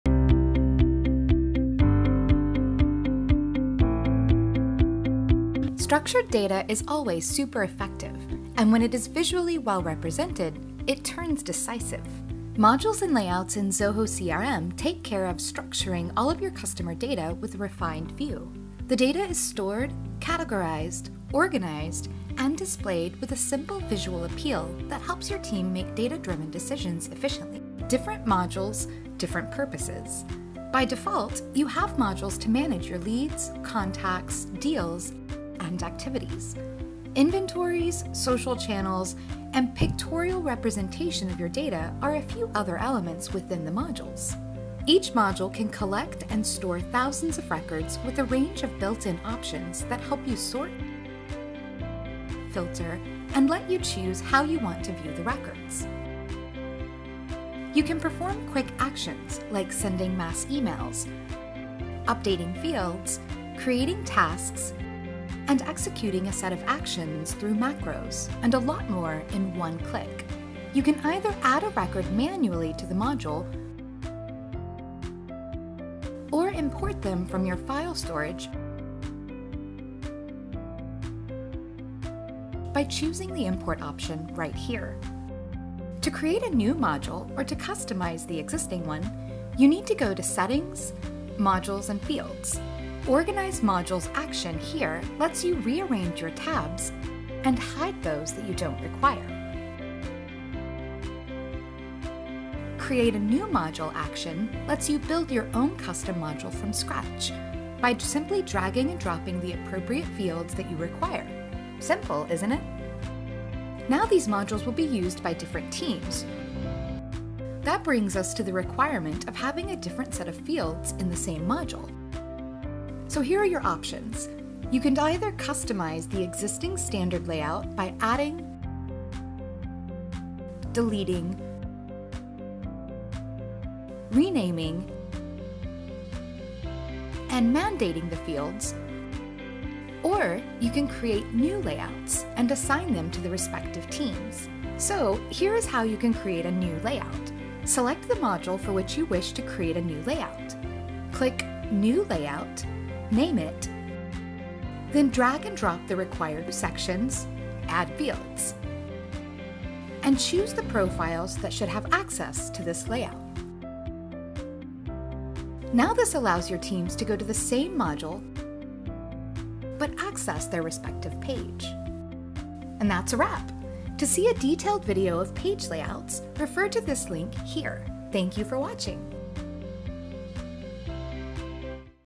Structure CRM data using Modules and Layouts | Zoho CRM in a Nutshell
Create, edit and customize your CRM modules and layouts, with ease! In this video, you will learn about the benefits of organizing your CRM database based on your organizational requirements using Modules and Layouts. While there are multiple ways of CRM data representation, why not start with something practical and easy to use.

Covered in this video are numerous uses of Modules and Layouts essentially designed to enhance customer service, such as:
▫️Pictorial representation of CRM data
▫️Collecting and storing a large number of records within your modules
▫️Customizing the way you view the records
▫️Performing quick actions like sending mass emails, updating fields, creating tasks and executing a set of actions, and a lot more in one click
▫️Building your own custom modules using simple drag and drop actions making these freshly curated modules available for teams to use
▫️Similarly, you can customize existing layouts or build another from scratch and perform the above-mentioned actions.